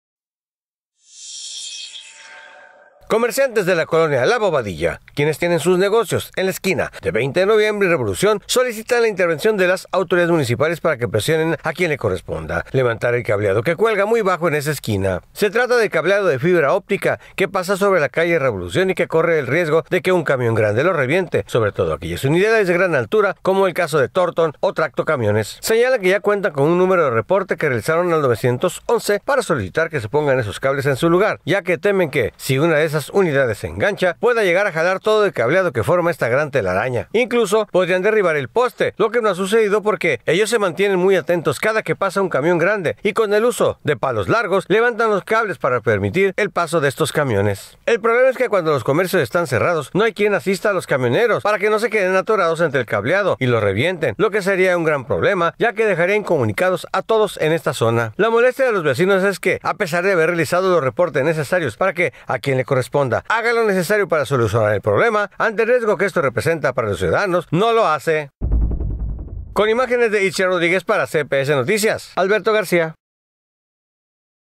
Cables de telefonía cuelgan sobre la calle Revolución | CPS Noticias Puerto Vallarta
Comerciantes de la colonia Bobadilla, quienes tienen sus negocios en la esquina de 20 de noviembre y Revolución, solicitan la intervención de las autoridades municipales, para que presionen a quien le corresponda levantar el cableado que cuelga muy bajo.